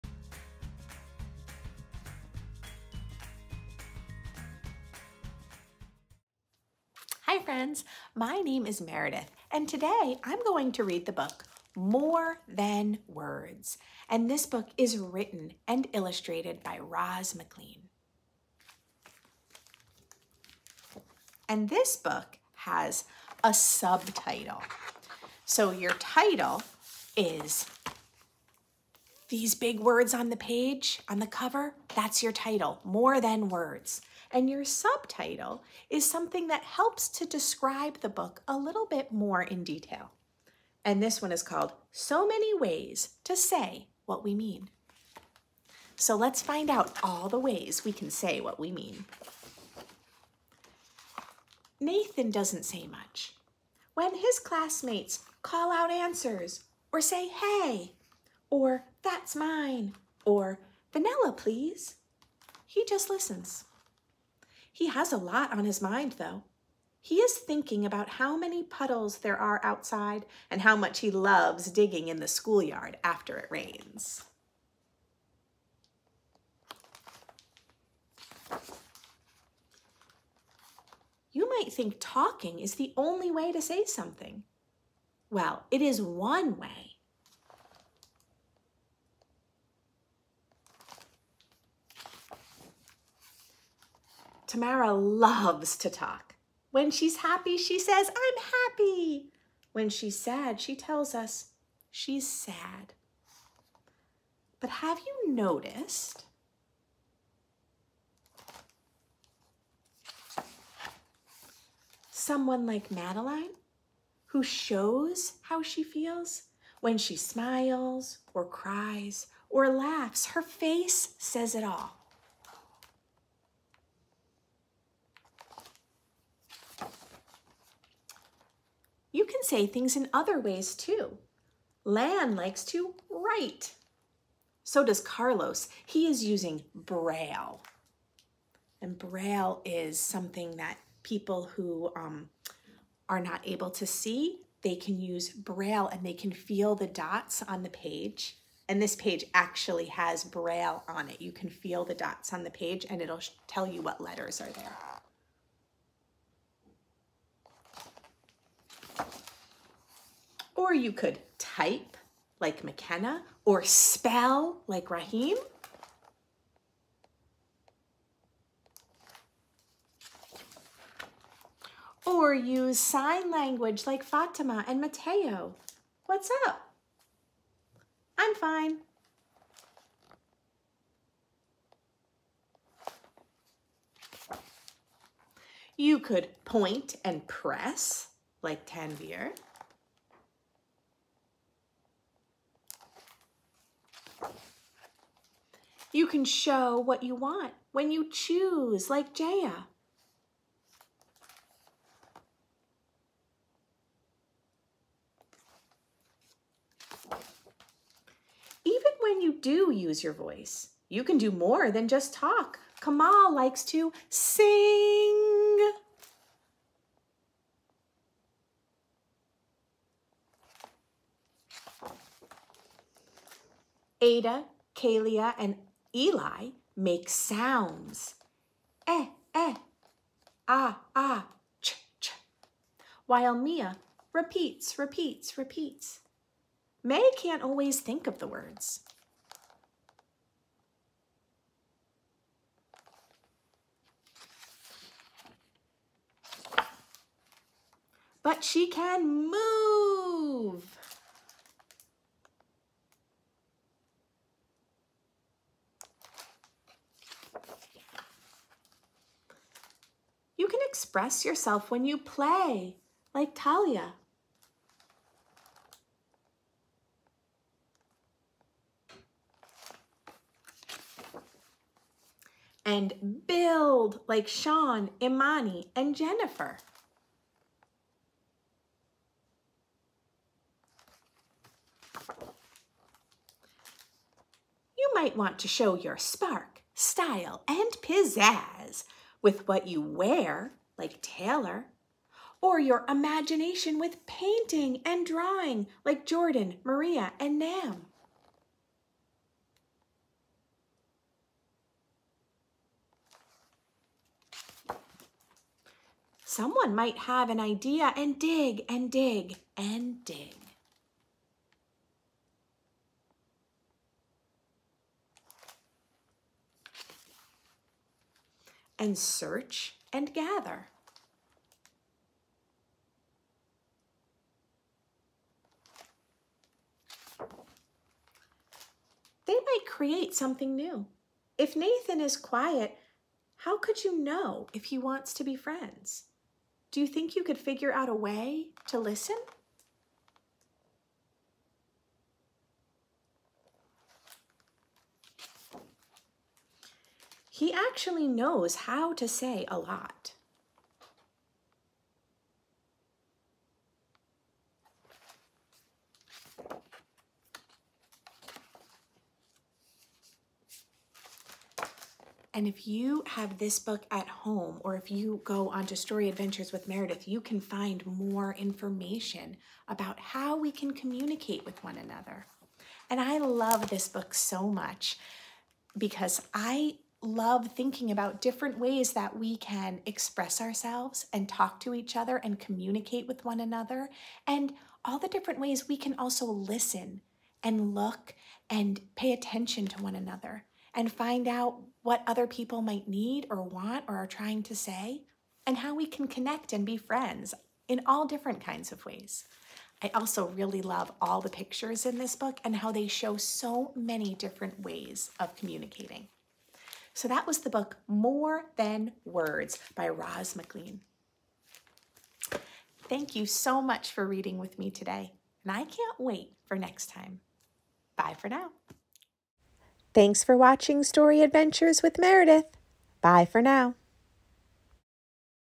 More Than Words Preschool Learning Video Kindergarten Picture Book Read Aloud
More Than Words: So Many Ways To Say What We Mean by Roz MacLean. Published by Henry Holt & Co. 2023.

A favorite picture book of 2023. Such a lovely book describing all the ways we can communicate with each other even without words. Wonderful for social-emotional development and inclusivity.